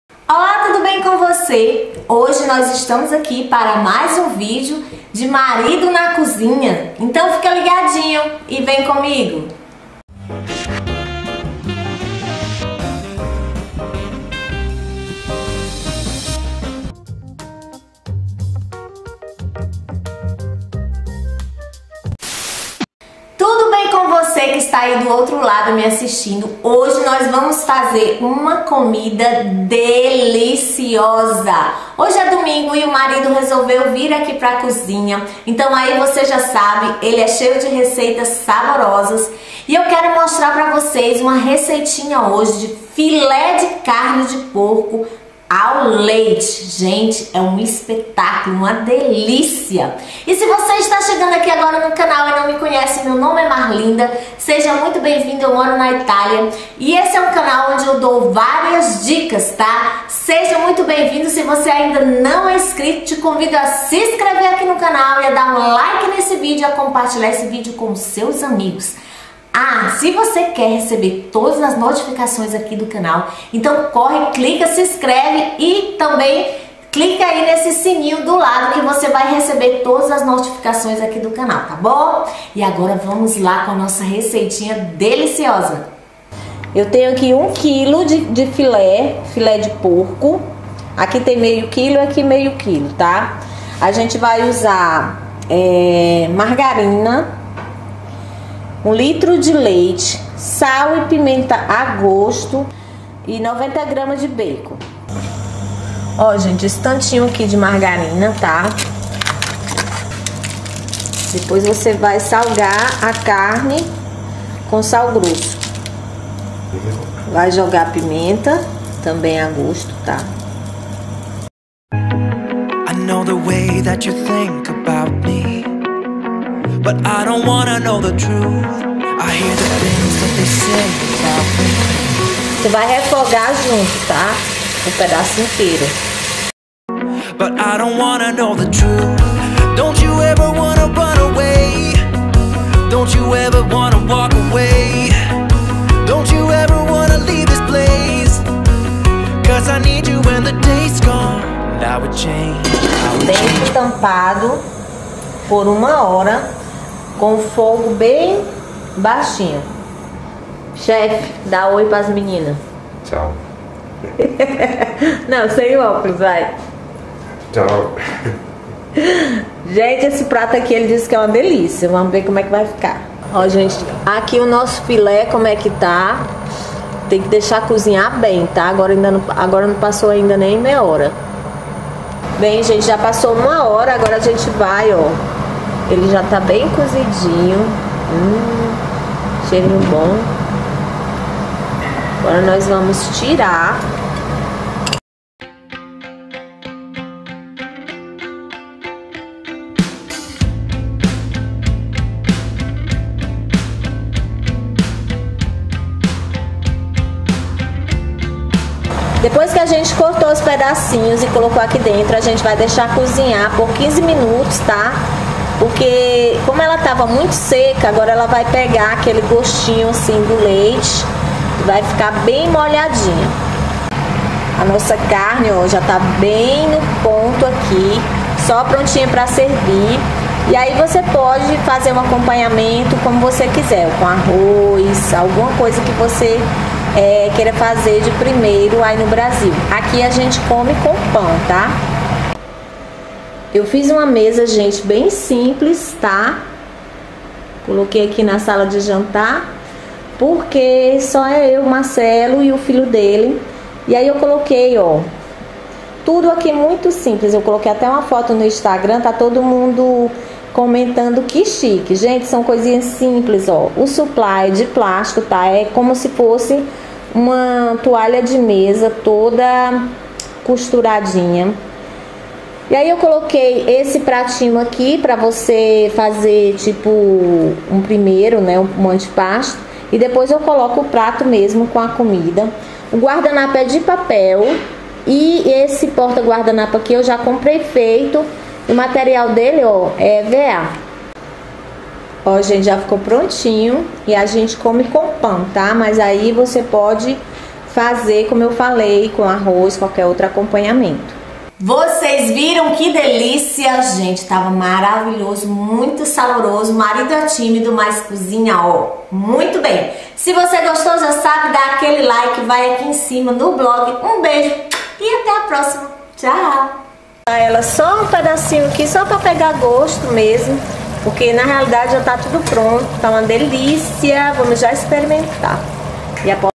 MARIDO NA COZINHA, FILé DE CARNE DE PORCO AO LEITE, MARLINDA CANONICO
Marido na Cozinha, Filé de carne de porco ao leite.

💛💛💛Ingredientes:💛💛💛

1 Kg de filé de carne de porco
90 g de bacon
50g de margarina
Sal e pimenta a gosto
1L de leite
2 ou 3 folhas de Savia

💛💛💛Assista Também💛💛💛